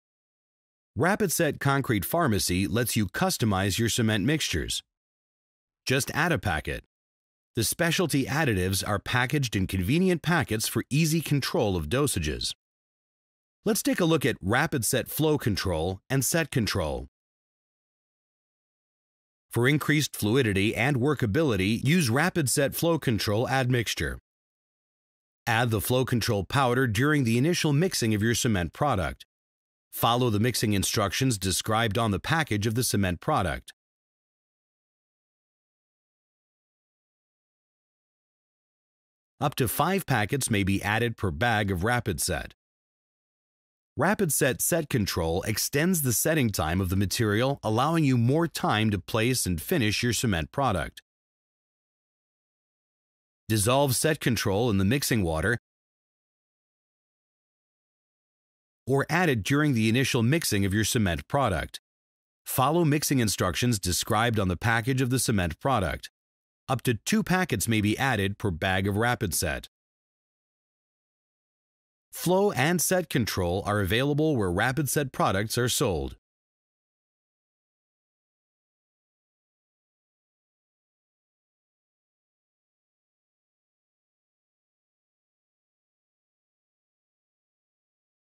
Rapid Set® Concrete Pharmacy®
Rapid Set® SET CONTROL® slows the setting time of Rapid Set® giving you more time to place and finish your mixture. Set Control® also works to retard cement in warm weather.
Rapid Set® FLOW CONTROL® increases the flowability of the Rapid Set® mixture for when a fluid consistency is required.